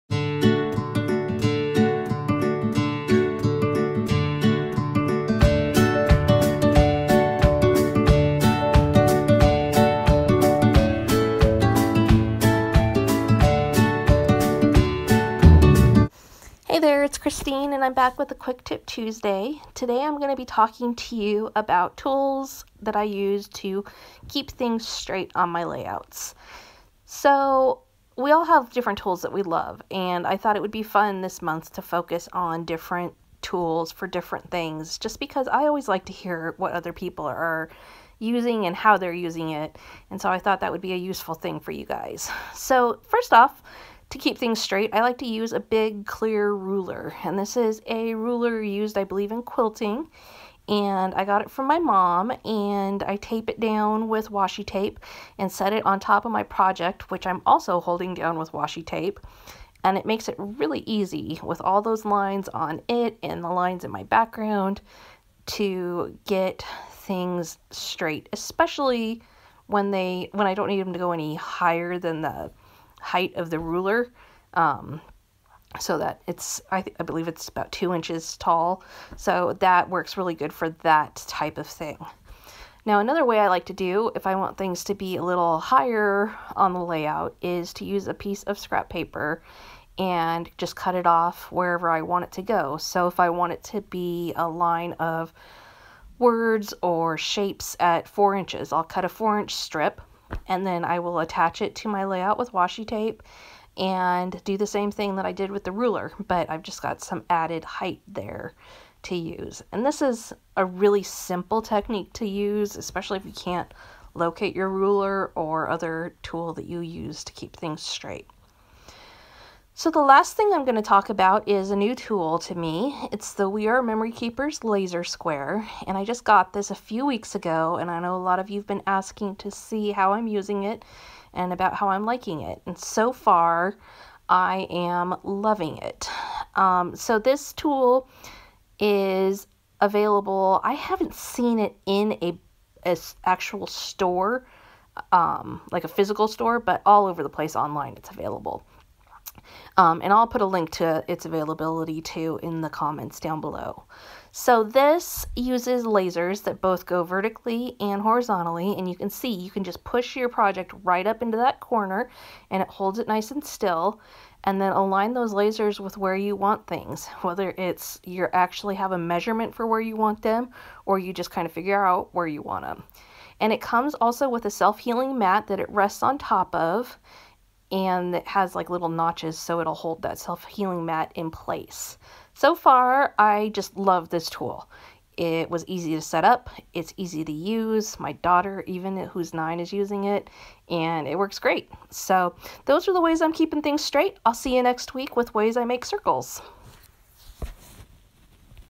Quick Tip Tuesday #10 - Keeping Things Straight
This month's Quick Tip Tuesday is going to focus on different tools I use in my paper crafting and scrapbooking creations.  Today I am sharing how I keep things straight on my layouts and sharing my new favorite the We R Memory Keepers Laser Square!